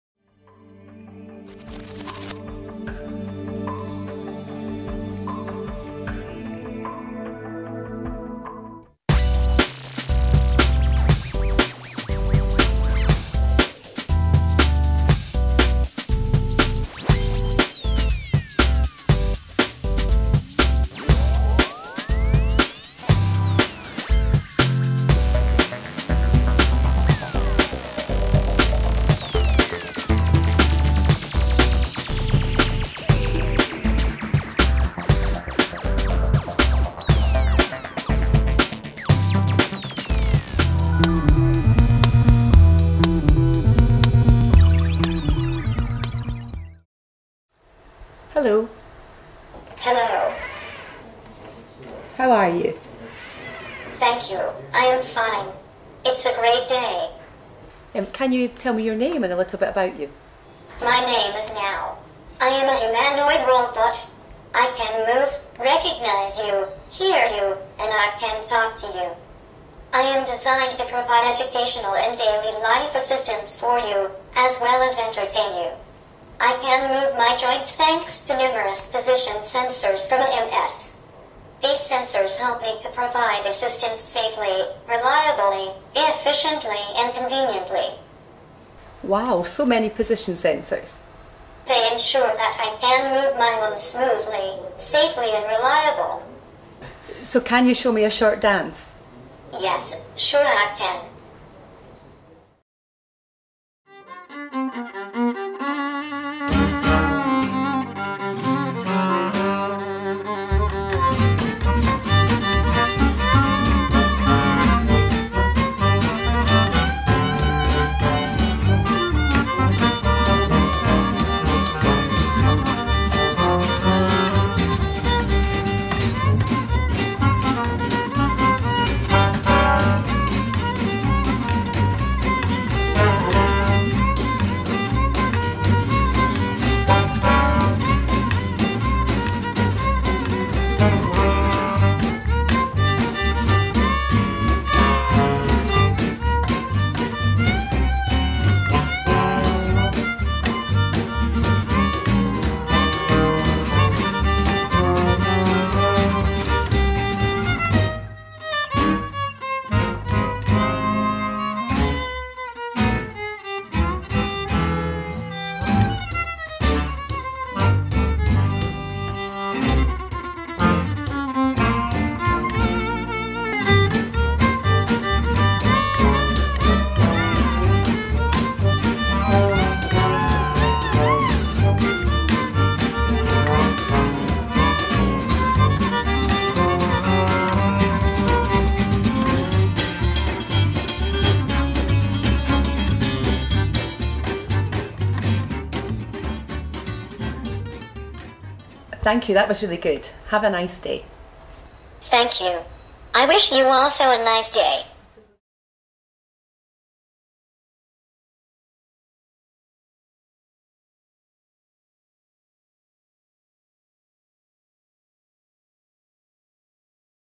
Contactless Magnetic Position Sensors in Robotics
High-speed position sensors with build-in magnetic strayfield immunity deliver the ideal solution for fine robot joint motor control.